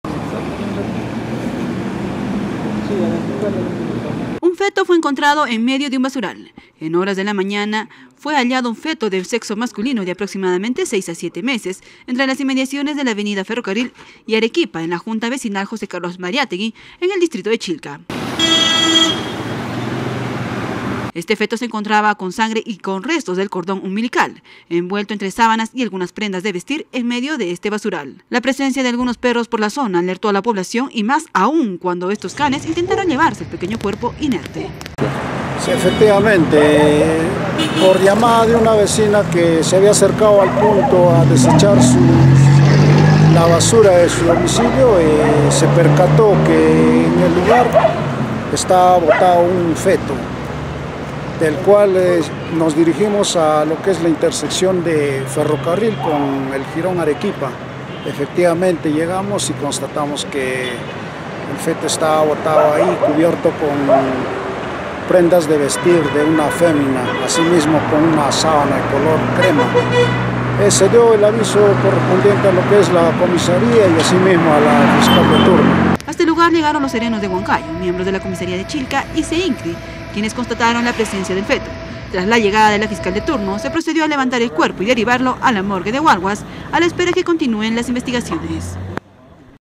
Huancayo: Un feto es hallado en medio de basural
Un feto fue encontrado en medio de un basural. En horas de la mañana fue hallado un feto de sexo masculino de aproximadamente 6 a 7 meses entre las inmediaciones de la avenida ferrocarril y Arequipa en la Junta Vecinal José Carlos Mariátegui en el distrito de Chilca.